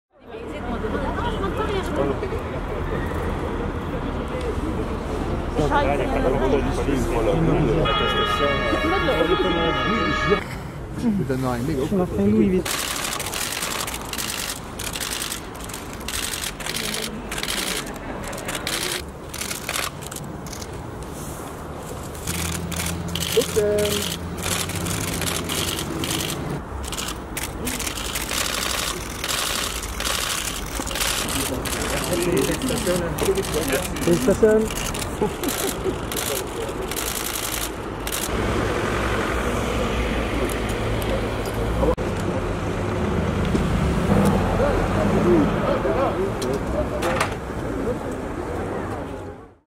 Charles, 3 jours, est sorti de la maternité
LUXEMBOURG - Le couple grand-ducal héritier a présenté ce mercredi à la presse son fils Charles, âgé de trois jours, à la sortie de la maternité du CHL.